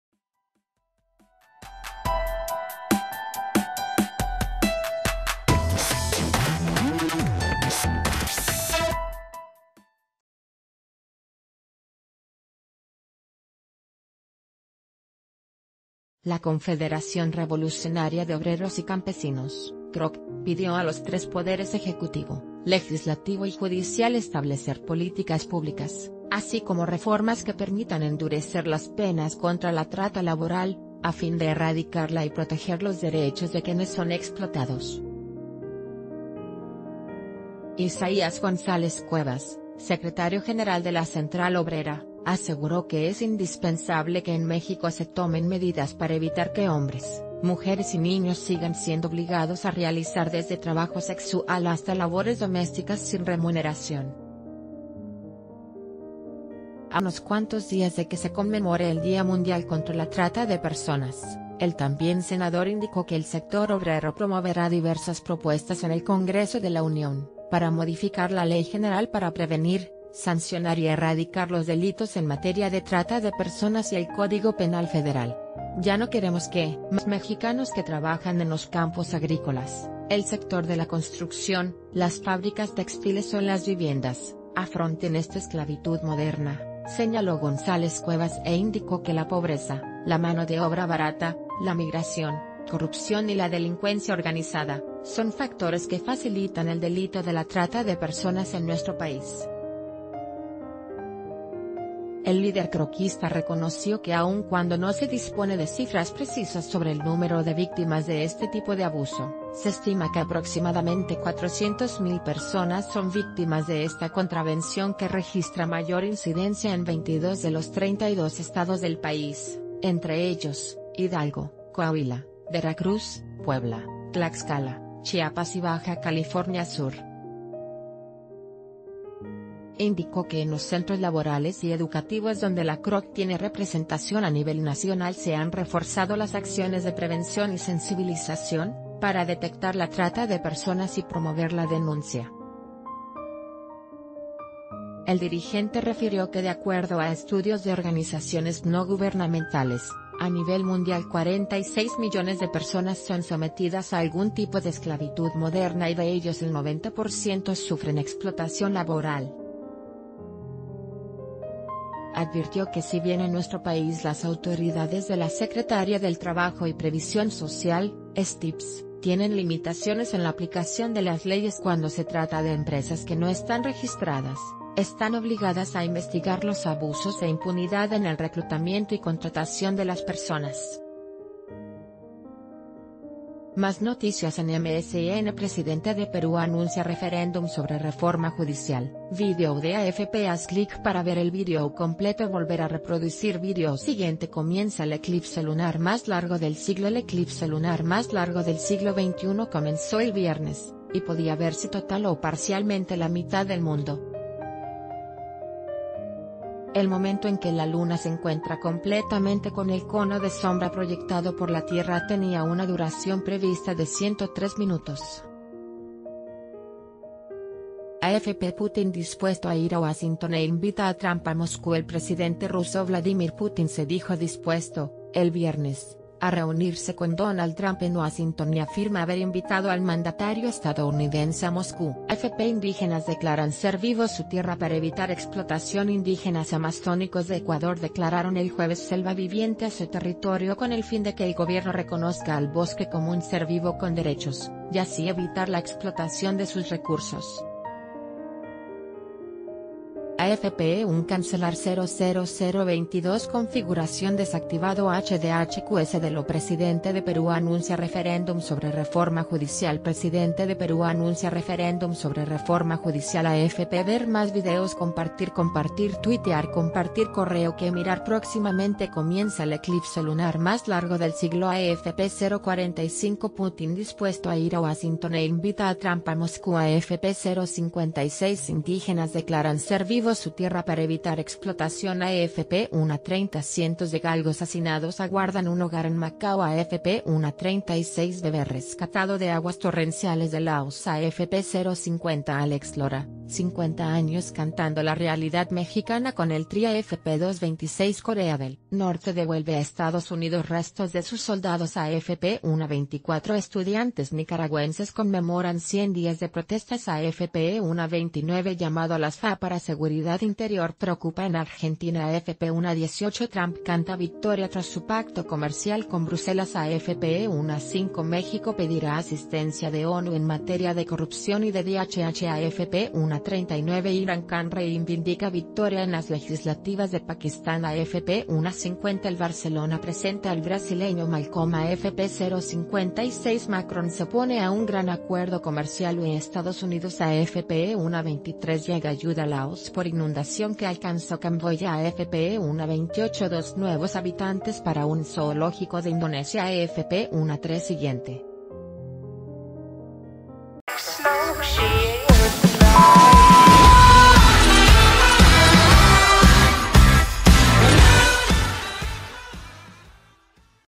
Pide CROC endurecer penas contra trata laboral
La Confederación Revolucionaria de Obreros y Campesinos (CROC) pidió a los tres poderes ejecutivo, legislativo y judicial establecer políticas públicas, así como reformas que permitan endurecer las penas contra la trata laboral, a fin de erradicarla y proteger los derechos de quienes son explotados.